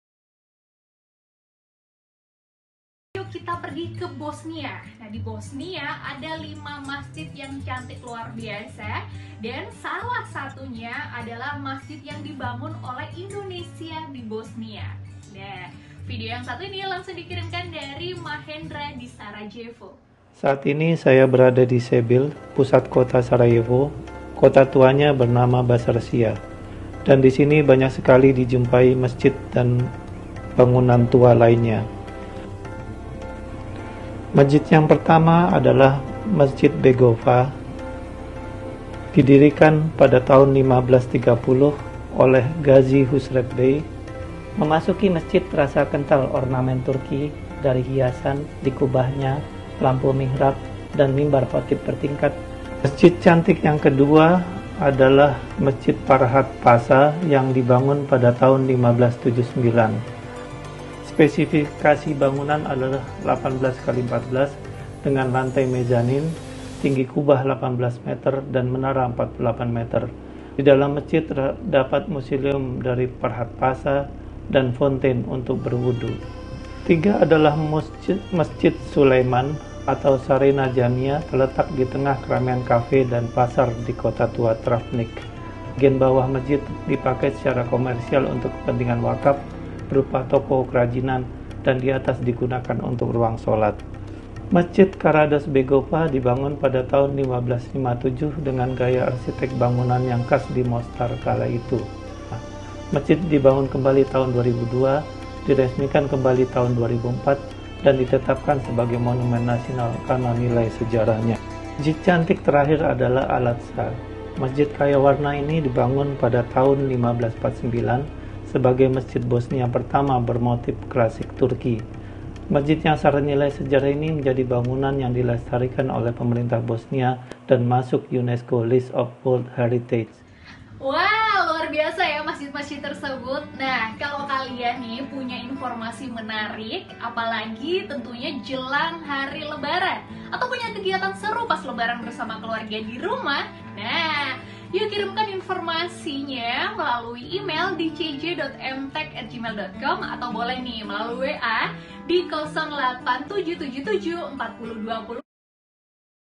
Capturing berita tentang mesjid di Bosnia di Vidio breaking news
Ini adalah tayangan khusus covid 19 dalam vidio breaking news pada tgl 22/05/20 menayangkan 5 mesjid cantik di bosnia. berikut ini linknya